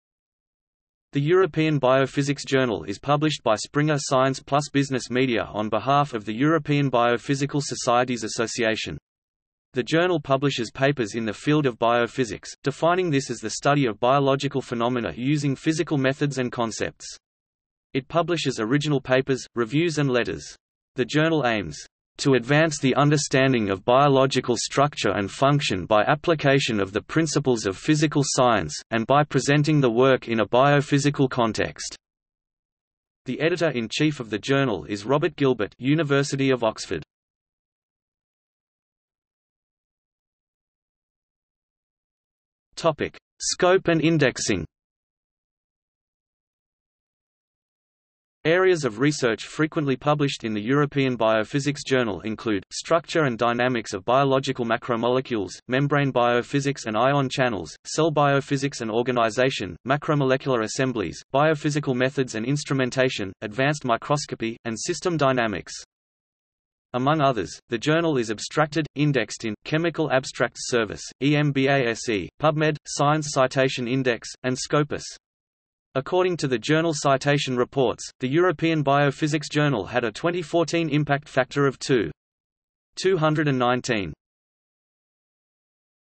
European Biophysics Journal | Wikipedia audio article
00:00:39 Scope and indexing

- Socrates

SUMMARY
The European Biophysics Journal is published by Springer Science+Business Media on behalf of the European Biophysical Societies Association. The journal publishes papers in the field of biophysics, defining this as the study of biological phenomena using physical methods and concepts. It publishes original papers, reviews and letters. The journal aims "to advance the understanding of biological structure and function by application of the principles of physical science, and by presenting the work in a biophysical context". The editor-in-chief of the journal is Robert Gilbert (University of Oxford).